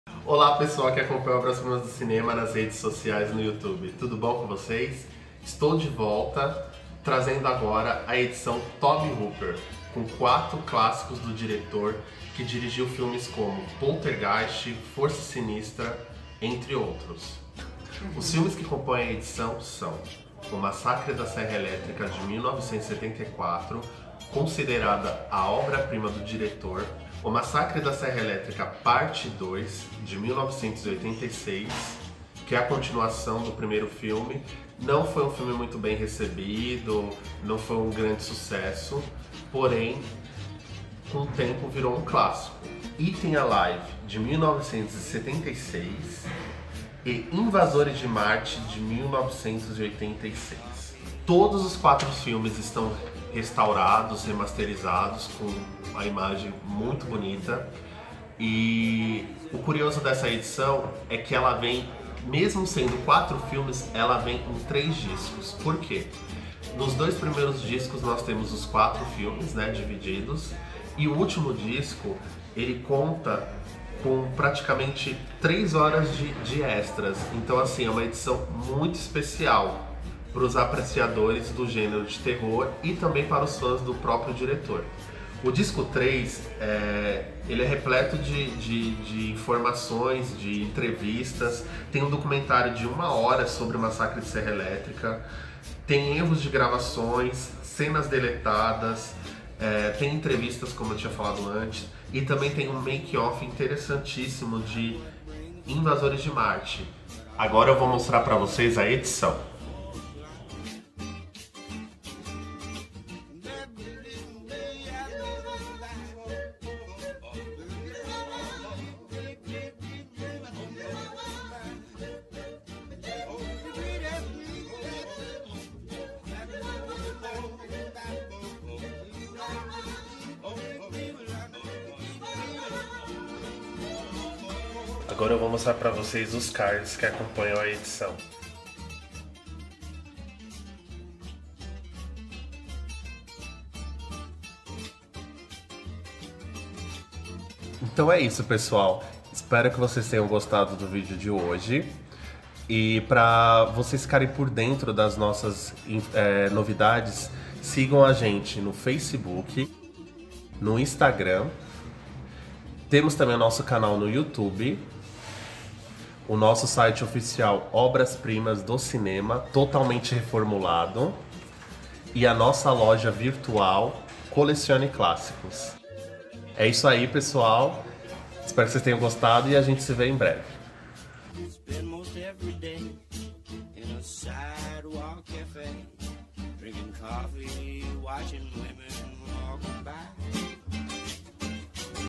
Unboxing - Tobe Hooper
Dando continuidade ao nosso "Outubro Sangrento", estamos de volta com mais um Unboxing e dessa vez iremos mostrar os detalhes da Coleção Tobe Hooper! Uma caixa que reúne 4 clássicos do diretor, indispensáveis para qualquer fã do diretor e do gênero de terror.